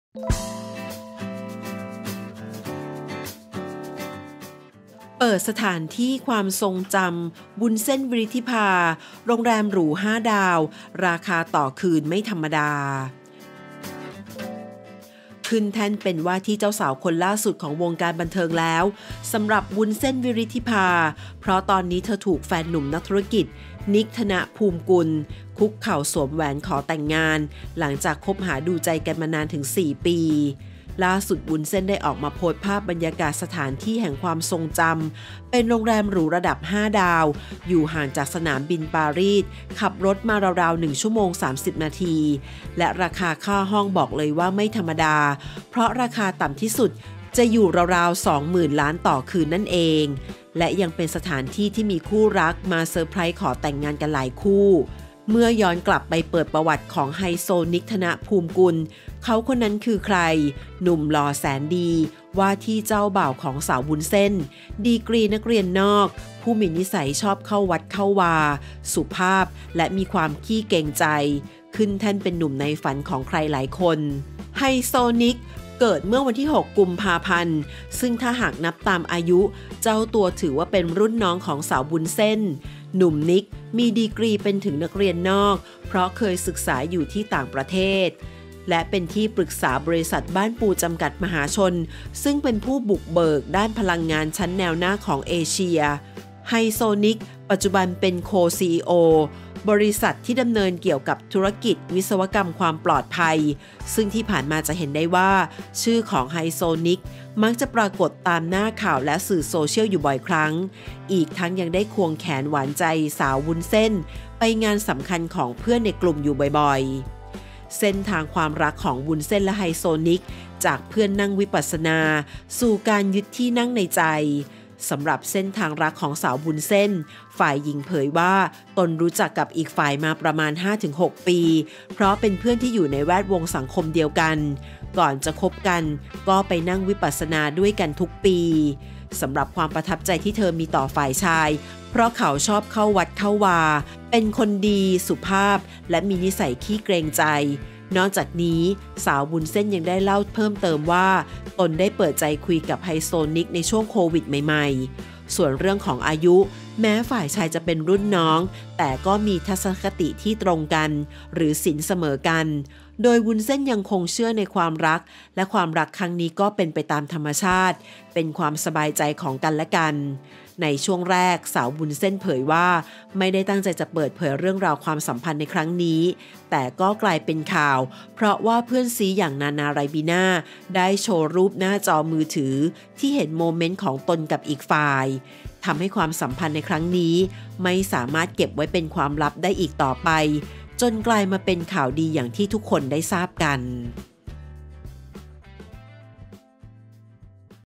ราคานี้ ไม่รวยจริงทำไม่ได้! เปิดสถานที่ความทรงจำ ‘วุ้นเส้น วิริฒิพา’ โรงเเรมหรู ราคาต่อคืนไม่ธรรมดา
ราคานี้ ไม่รวยจริงทำไม่ได้! เปิดสถานที่ความทรงจำ ‘วุ้นเส้น วิริฒิพา’ โรงเเรมหรู 5 ดาว ราคาต่อคืนไม่ธรรมดา

ขึ้นเเท่นเป็นว่าที่เจ้าสาวคนล่าสุดของวงการบันเทิงเเล้ว สำหรับ “วุ้นเส้น-วิริฒิพา ภักดีประสงค์” เพราะตอนนี้เธอถูกเเฟน

หนุ่มนักธุรกิจ “นิกม์ ธนะภูมิกุล” คุกเข่าสวมแหวนขอแต่งงาน หลังจากคบหาดูใจกันมานานถึง 4 ปี

ล่าสุด วุ้นเส้น ได้ออกมาโพสต์ภาพบรรยากาศสถานที่เเห่งความทรงจำ Domaine Les Crayres เป็นโรงเเรมหรูระดับ 5 ดาว